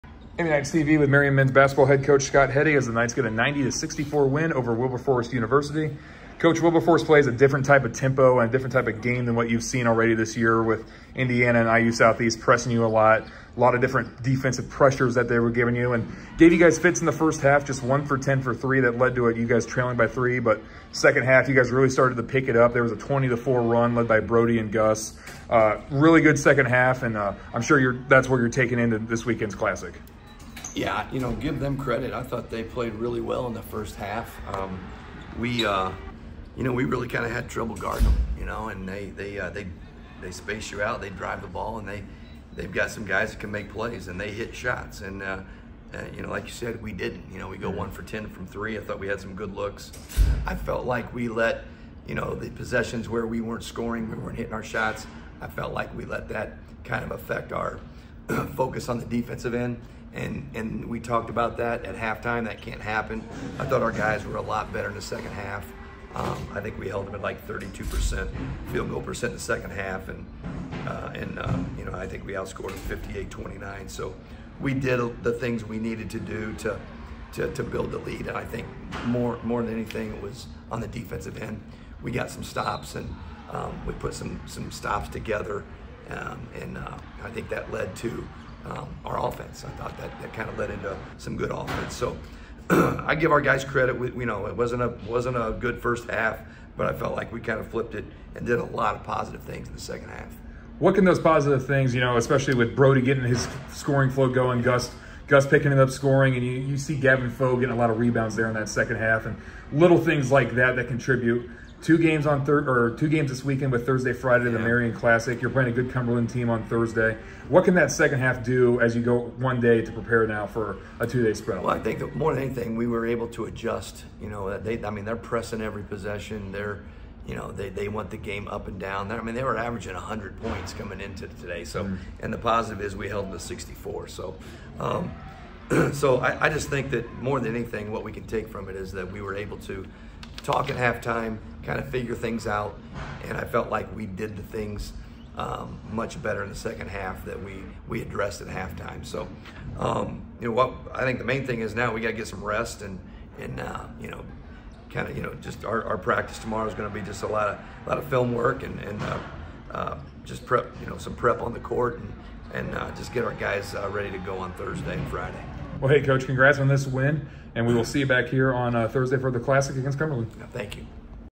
MBB Highlights vs Wilberforce
After trailing at the half on Tuesday night against Wilberforce, the Marian men's basketball team poured in 58 second half points to cruise to a 90-64 victory against Wilberforce. Coach Scott Heady shared his thoughts following the win with MUKnightsTV as the Knights open a three-game week.